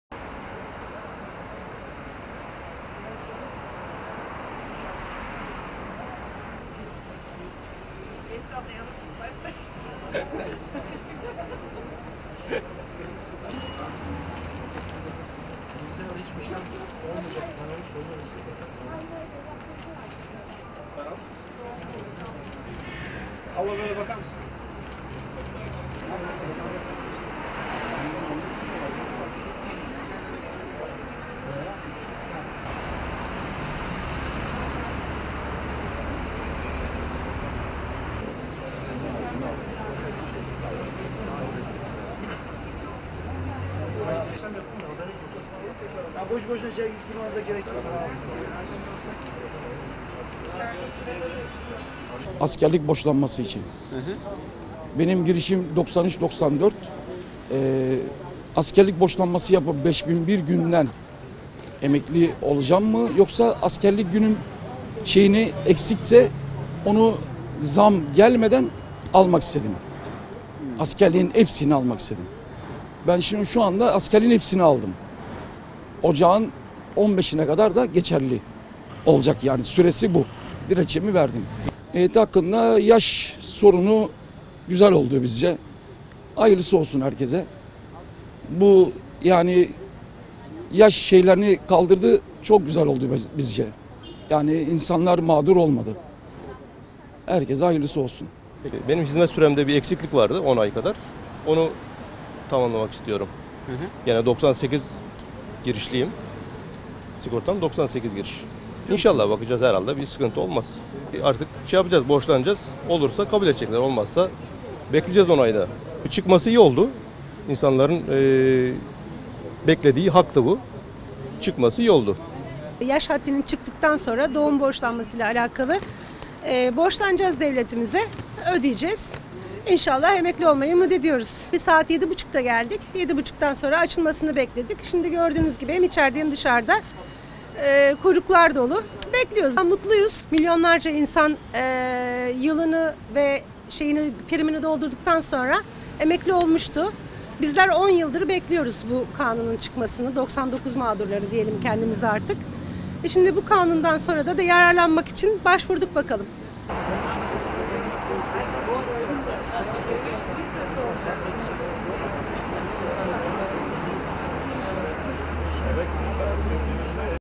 İzmir’de EYT kuyruğu
İzmir’de, emeklilikte yaşa takılanlarla (EYT) ilgili yapılan açıklama sonrası vatandaşlar, borçlanma ve prim gün sayısı hesaplaması için Sosyal Güvenlik Kurumuna (SGK) adeta akın etti